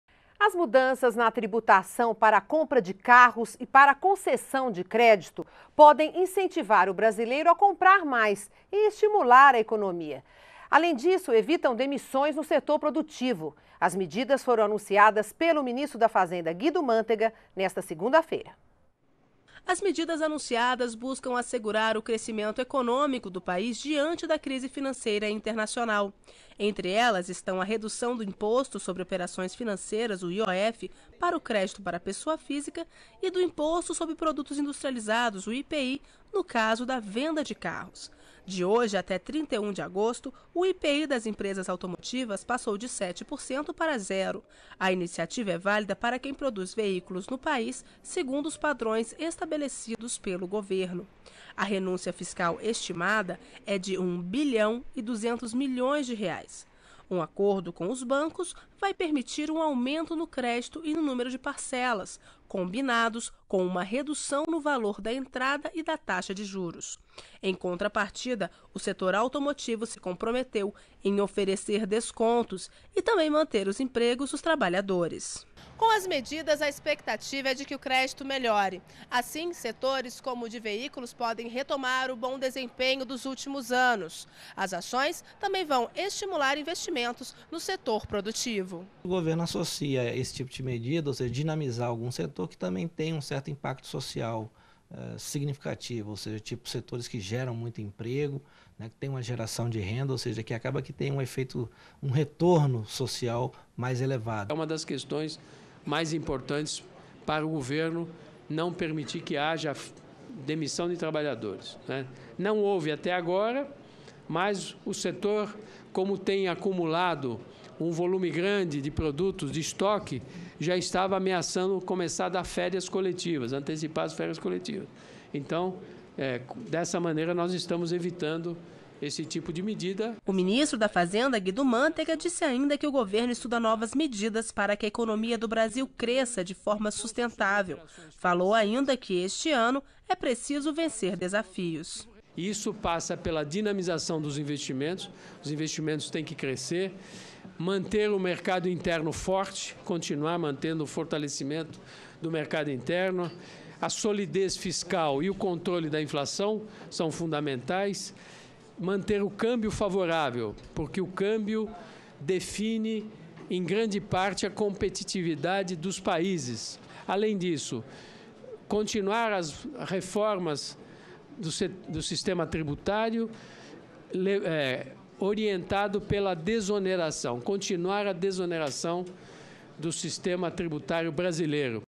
Governo federal anuncia novas medidas para estimular o crescimento econômico do país
NBR NOTÍCIAS - 22.05.12: O Ministro da Fazenda, Guido Mantega, anunciou nesta terça-feira (22) medidas do governo federal para estimular o crescimento econômico do país. Entre as mudanças, estão a redução de impostos para crédito de pessoas físicas e  na tributação para compra de carros.